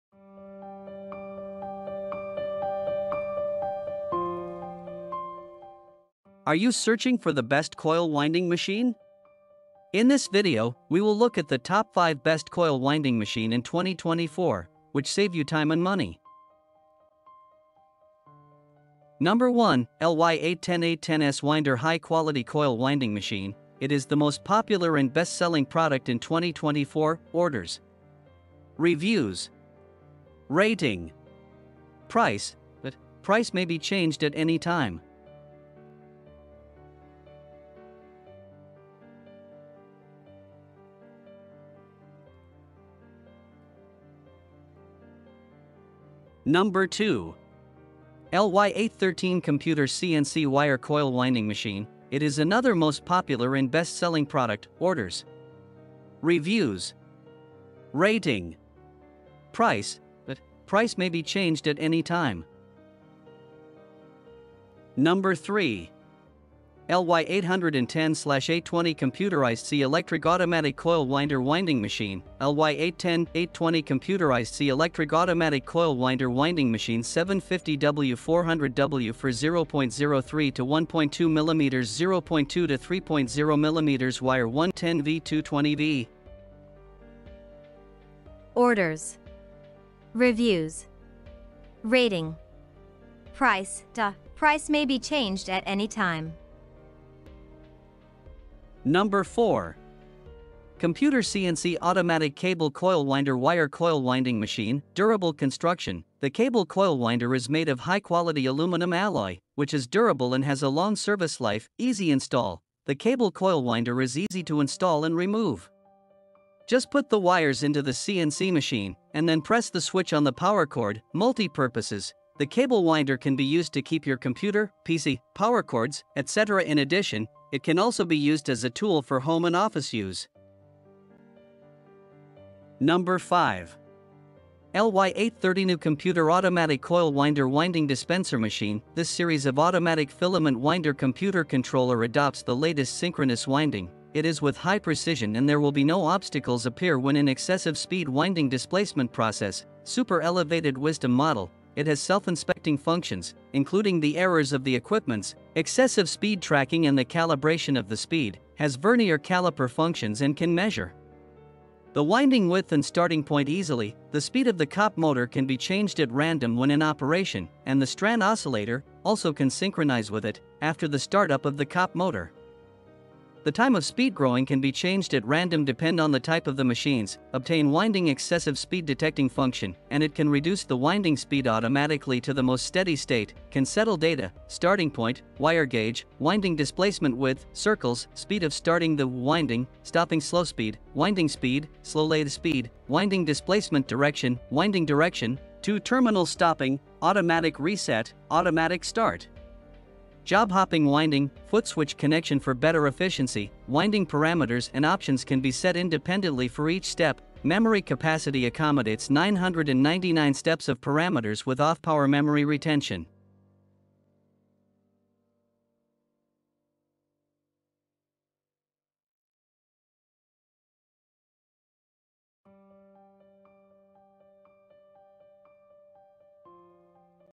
Best Coil Winding Machine In 2024! Top 5 Best Universal Coil Winding Machine
Best Coil Winding Machine In 2024! Top 5 Best Universal Coil Winding Machine On Aliexpress
Are you Searching for the Best Coil Winding Machine? In this video, We will look at the Top 5 Best Coil Winding Machine In 2024.

Top 1. LY 810 810S Winder High Quality Coil Winding Machine
Top 2. LY 813 Computer CNC Wire Coil Winding Machine
3. LY 810 /820 Computerized C Electric Automatic Coil Winder Winding Machine
4. Computer CNC Automatic Cable Coil Winder Wire Coil Winding Machine
5. LY 830 New Computer Automatic Coil Winder Winding Dispenser Machine